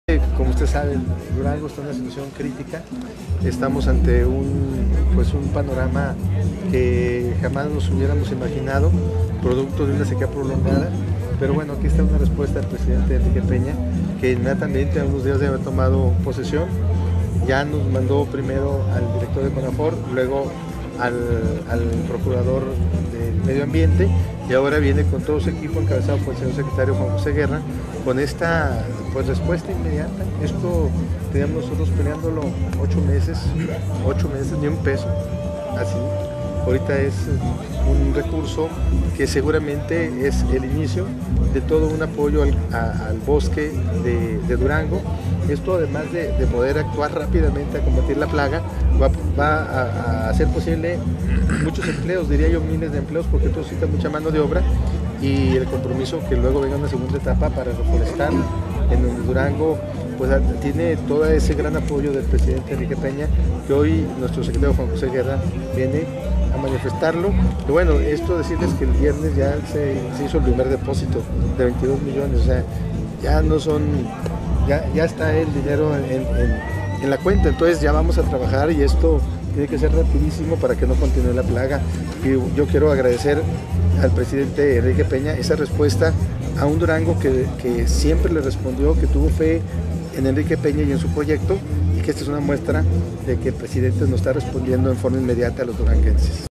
Jorge Herreda Caldera, JHC informa sobre la plaga en Dgo.
Menciona que Dgo está en una situación critica teniendo un panorama que jamás nos hubiéramos imaginado producto de una sequia